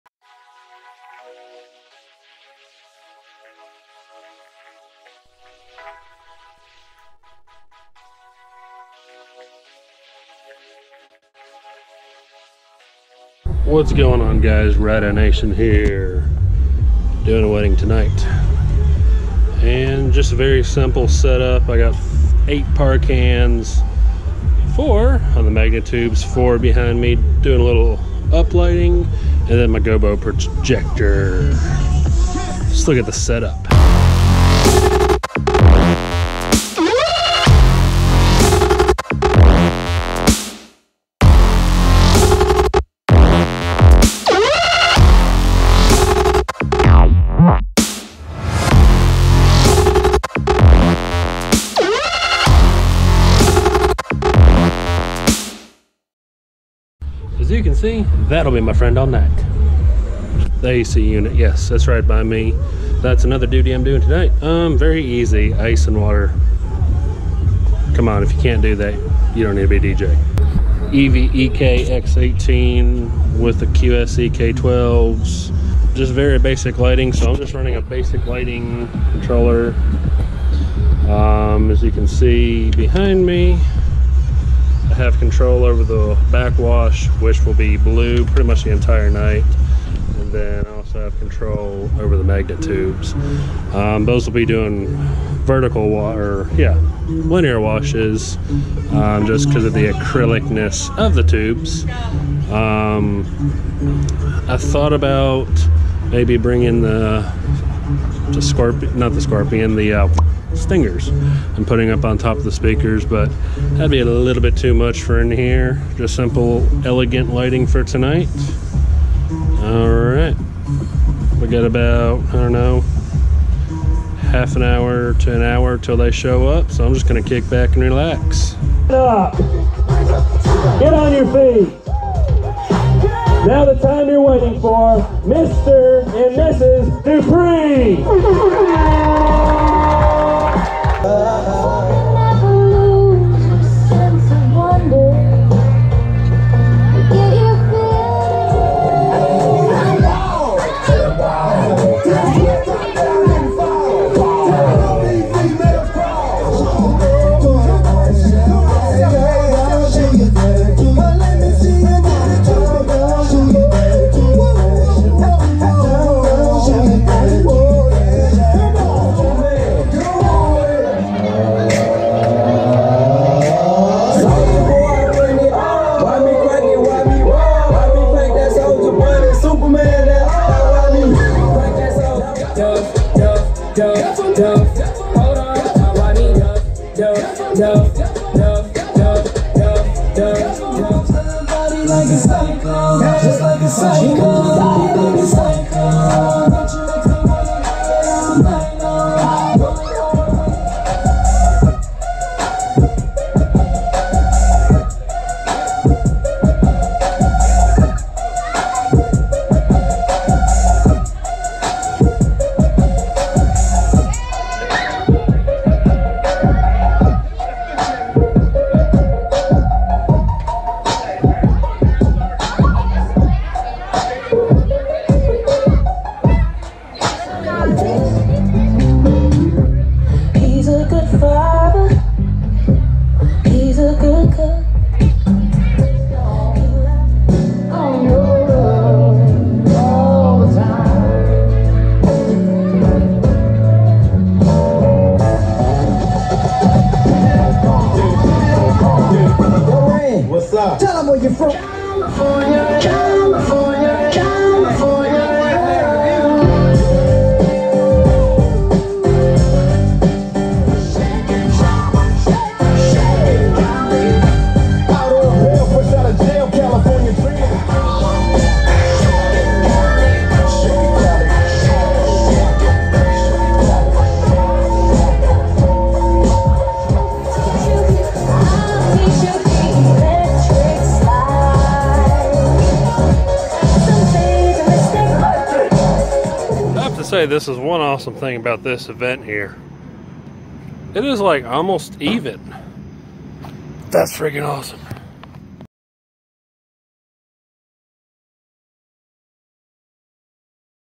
First Suit & Tie Wedding
This was a 2019 wedding for Autumn & Byron Dupre. I was hired for the reception only. I say my first suit and tie event because most weddings I DJ on a farm. This was in an event hall with AC.